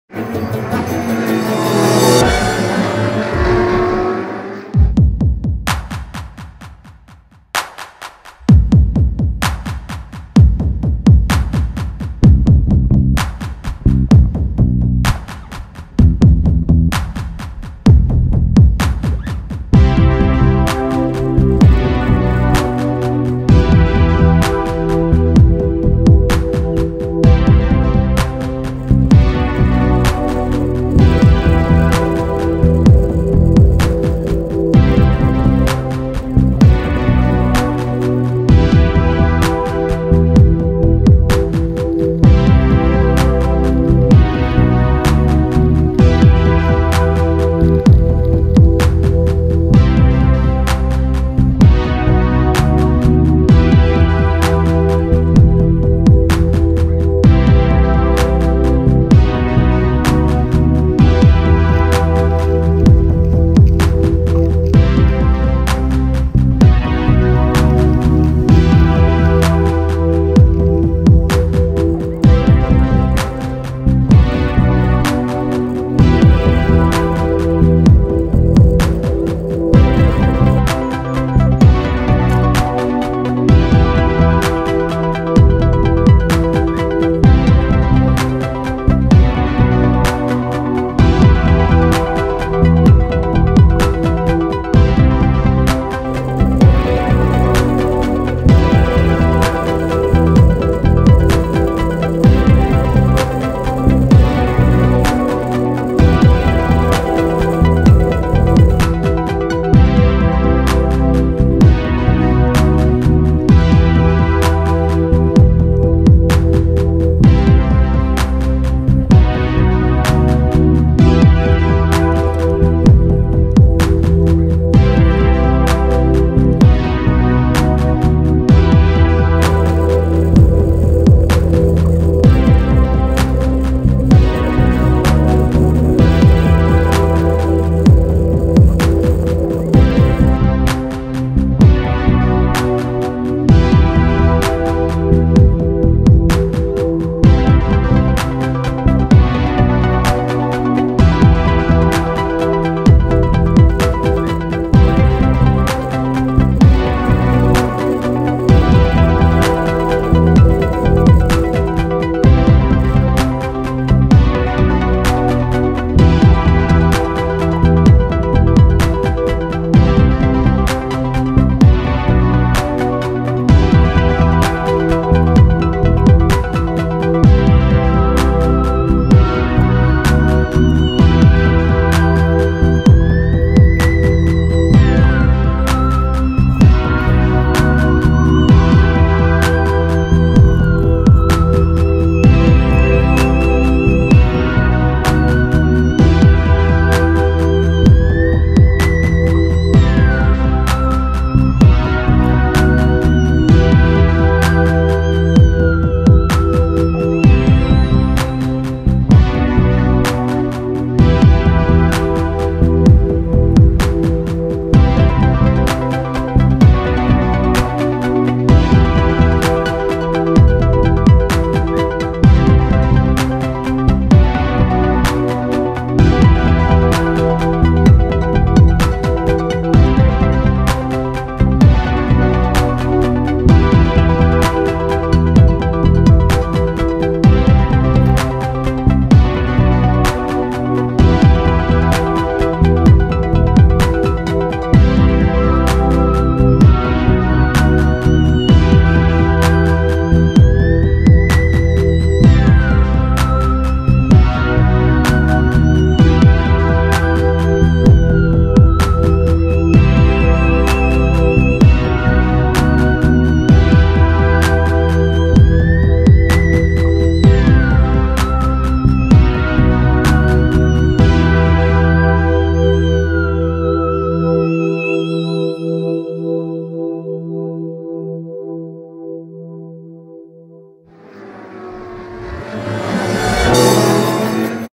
2014 Subaru Crosstrek Hybrid Iceland Expedition
2014 Subaru Crosstrek Hybrid Test Drive from drive from The Landmannalaugar Camp to Reykjavic during the Iceland Expedition for it´s global debut.